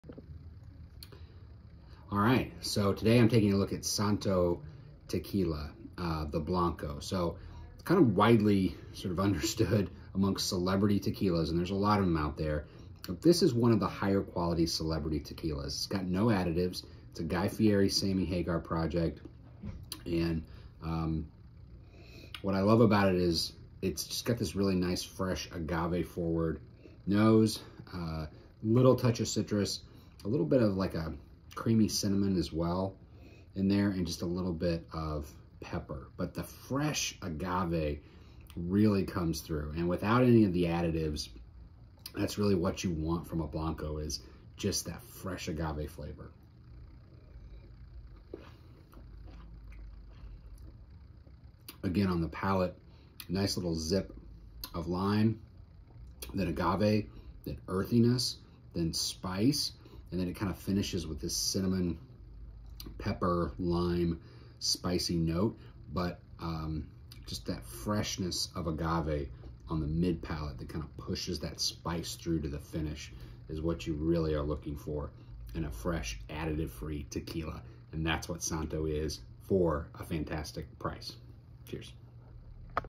Santo Blanco Tequila
Generally regarded by tequila fans as the BEST tasting "celebirty" tequila, Guy Fieri and Sammy Hagar's Santo Tequila comes from the relatively small 1107 NOM, producer of a slew of boutique and smaller brands via brick oven, stainless ferment, and small batch copper pot stills. In the case of Santo, NO ADDITIVES are employed, making it one of the most natural agave, fruit, and pepper tequila on the market. Nose is fresh agave with touches of cinnamon, lime, mineral and mint. On the palate, zesty citrus, mouth watering agave, and more subtle spice through the finish. Solid as hell, and sippable all day. 🙌🌵🍹 Best Celebrity Tequila Around